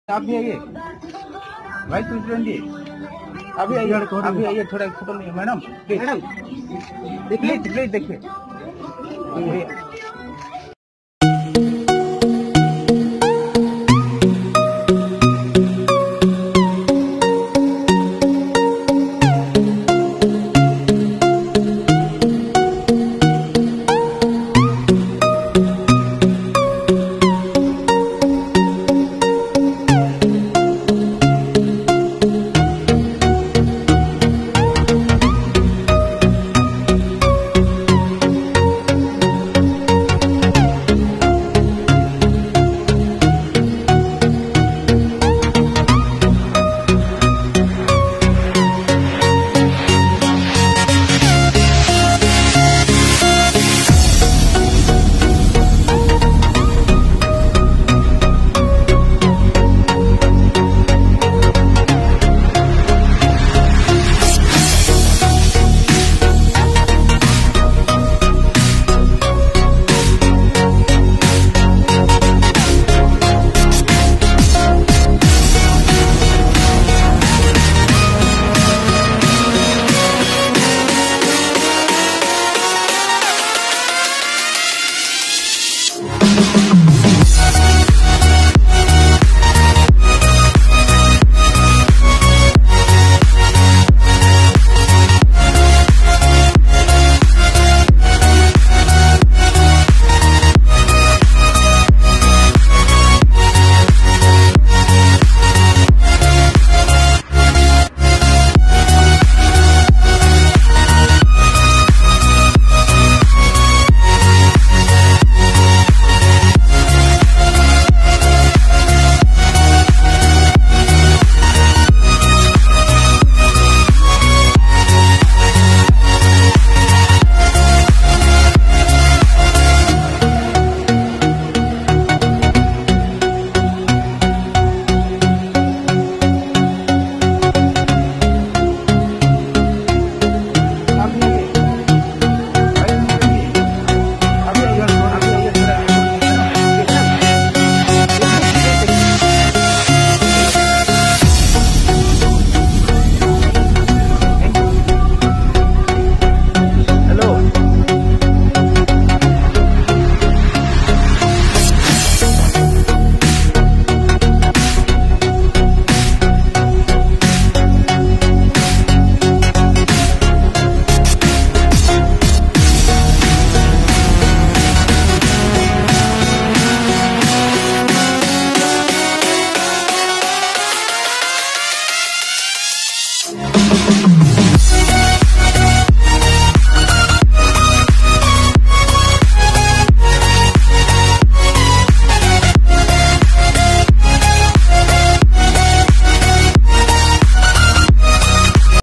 Beautiful Urvashi Routela in Sambalpur | Krish vlog
Hey guys .
This video is about the gorgeous Bollywood actress Urvashi Routela come to sambalpur Odisha  for opening ceremony of MAX fashion store.